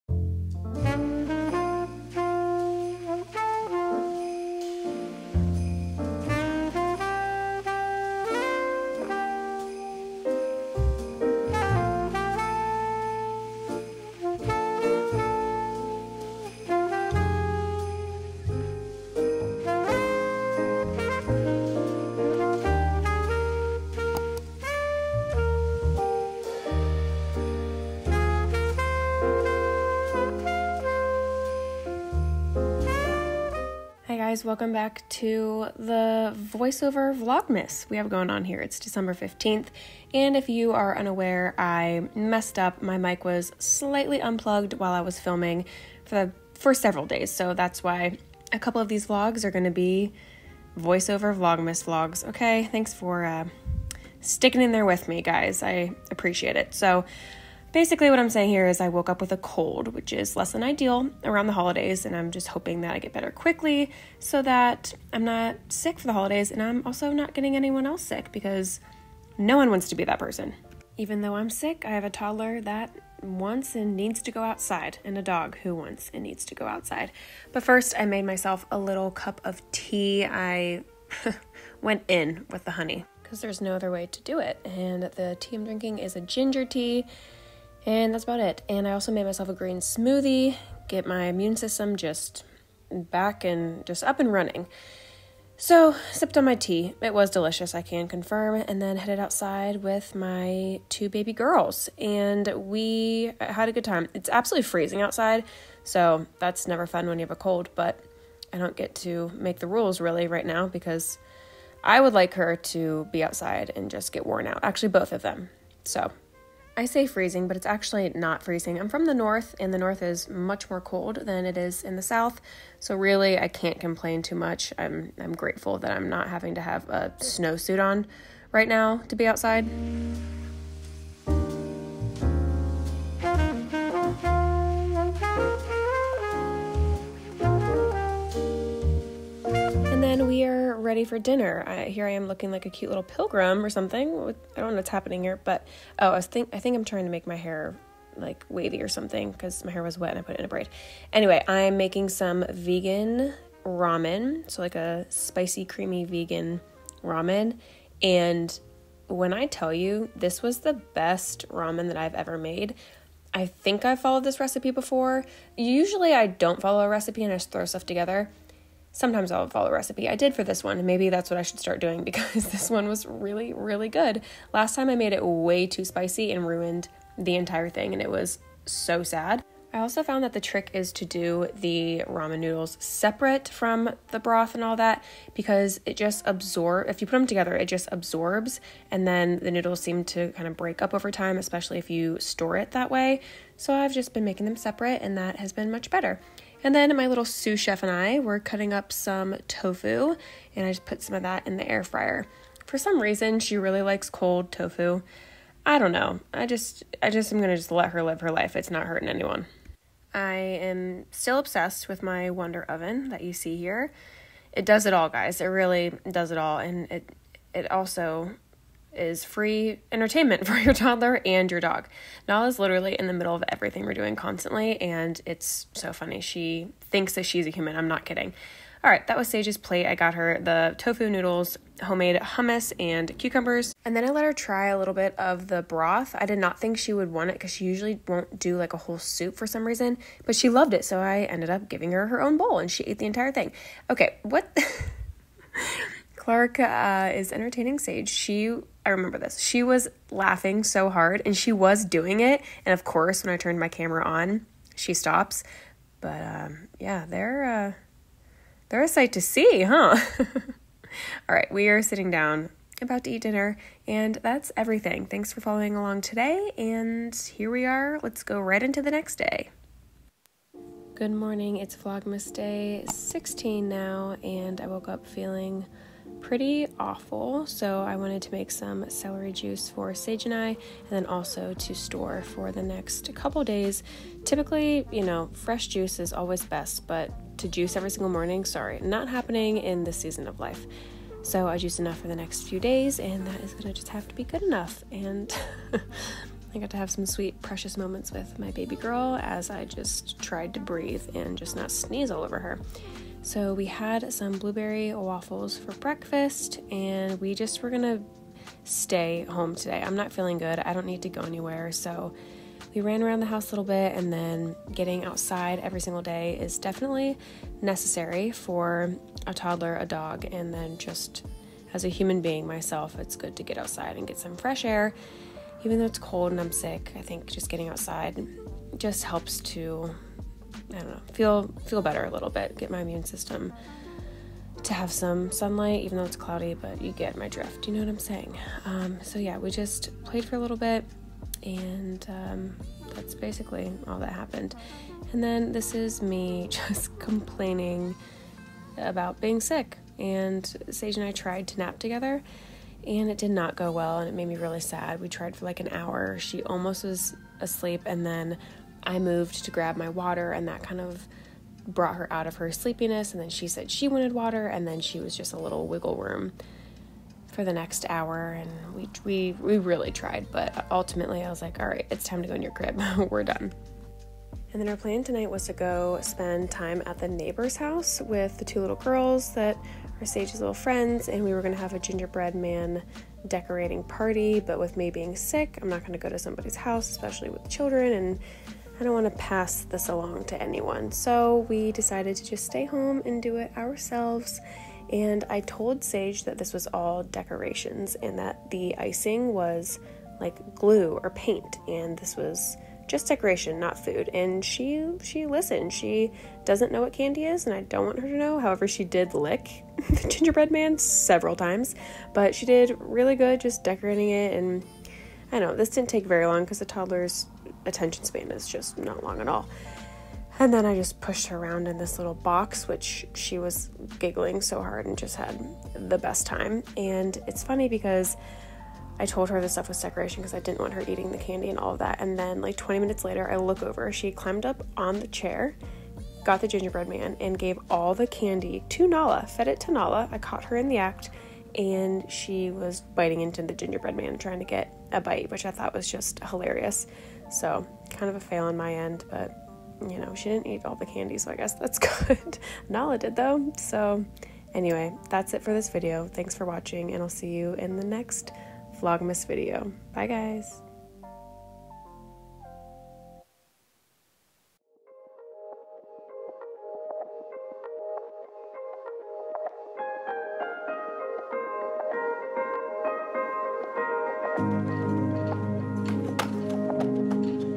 VLOGMAS DAY 15&16: im sick, vegan ramen, gingerbread man decorating
Happy Vlogmas!!! It’s day 15 & 16! Guys, my mic was unplugged for DAYS of vlogging. Do we like the voiceover?? Back to real audio soon!

Let me know if you need links to anything in the video.

DISCOUNTS

Code: BARTEE10

code: PLANTIFUL

code: plantifulalex

code: alex15